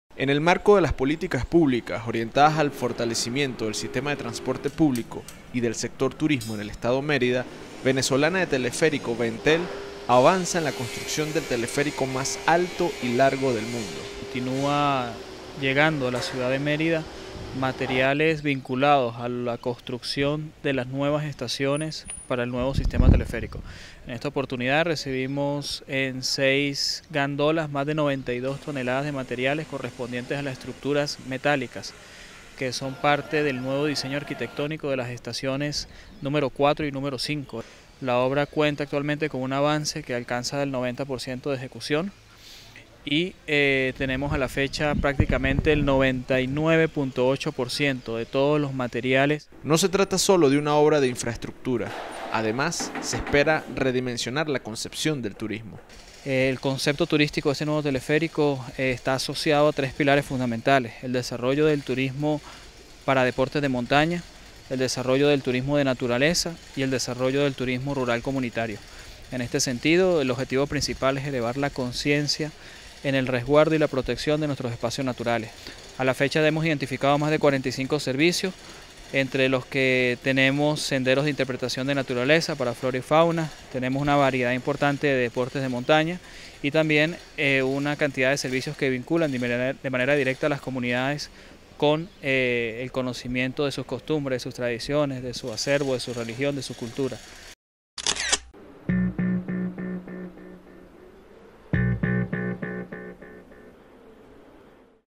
VENTEL avanza en la construcción del Teleférico Mukumbarí docx
Desde el Gobierno Bolivariano se avanza en la ejecución de políticas públicas que atienden el fortalecimiento del transporte público y nutren al sector turismo en el Edo. Mérida, desde el Sistema Teleférico de Mérida se avanza en la construcción del Teleférico más alto y largo del mundo, con la llegada continua de materiales que permitirán la culminación de tan importante obra, la cual, más allá de su magnitud en materia de infraestructura propondrá un nuevo concepto de turismo en Mérida.